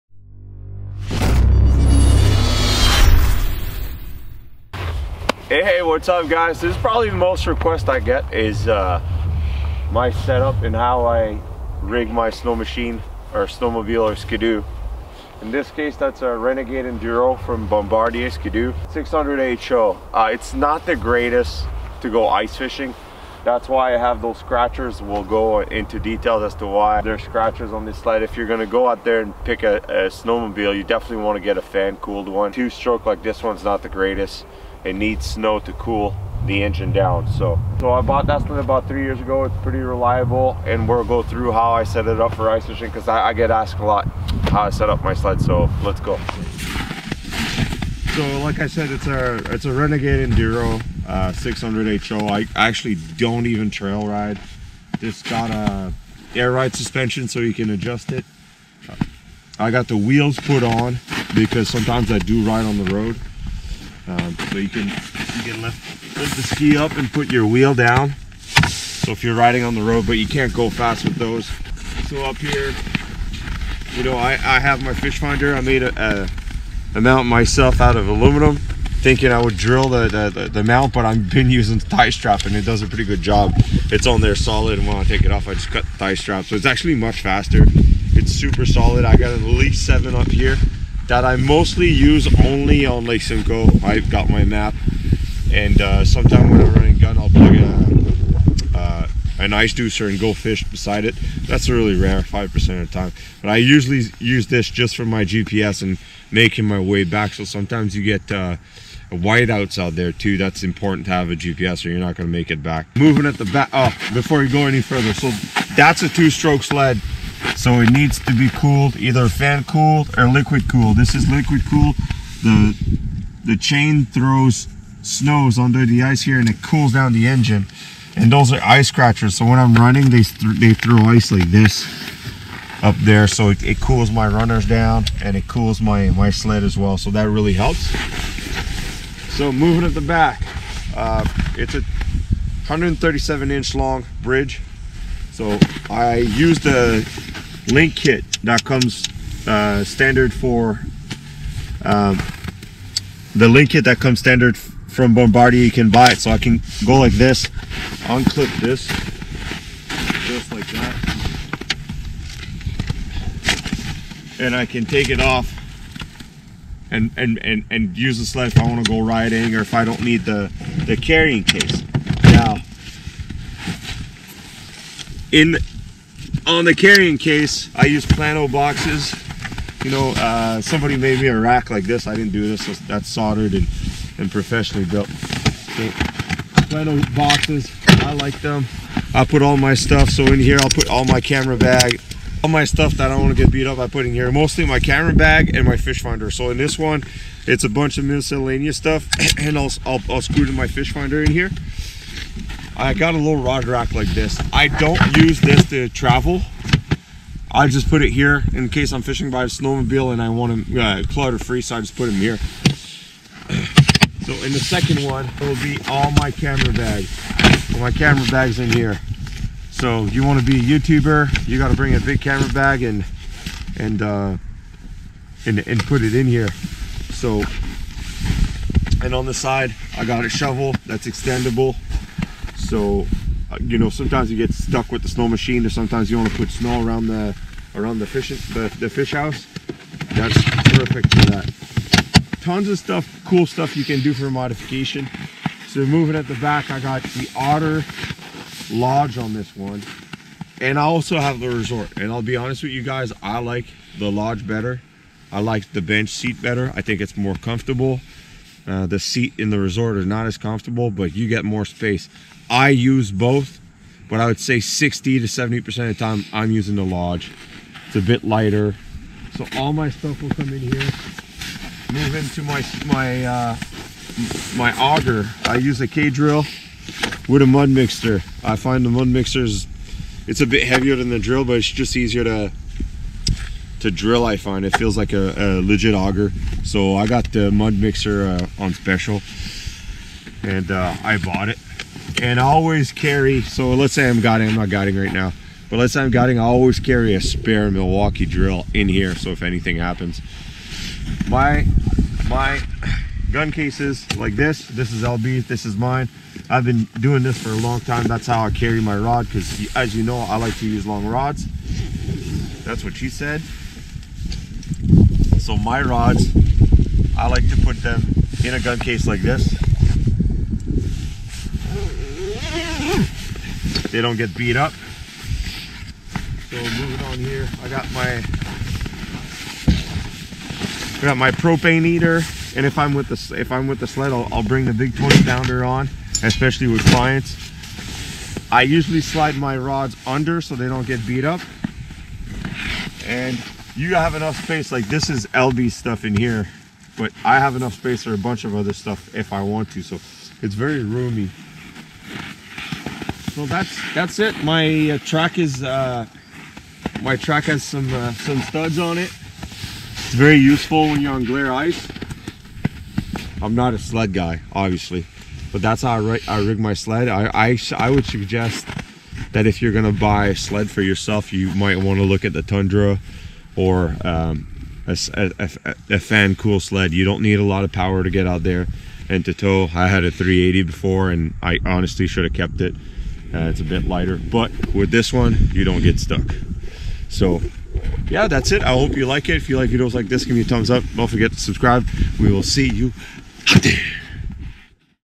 Ice Fishing Snowmobile Setup
Here is the review of my current setup asked by many.
2016 Renegade Enduro 600 h.o